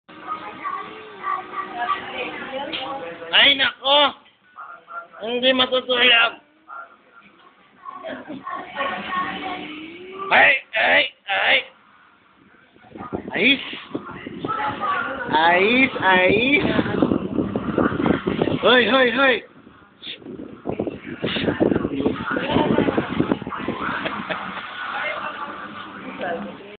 nang nagulat c budong
pagod na pagod at hapong-hapo ang ating bida na c budong pero wala pa rin makakapigil sa kanya para tayo ay pasayahin